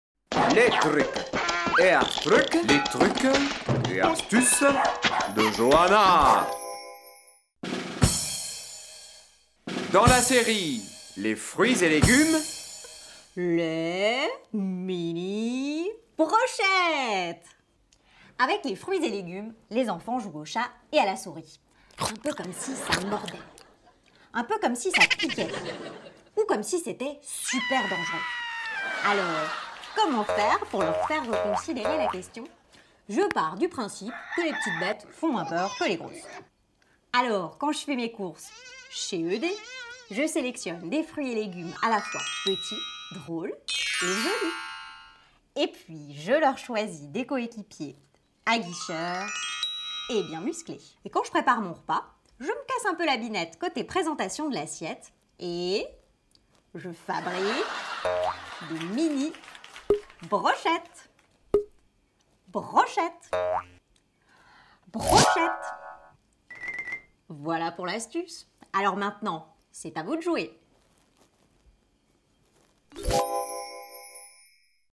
Des minis brochettes avec de la rosette !
Pour les enfants. Comment faire aimer les fruits et légumes aux enfants avec des brochettes et un peu dimagination ?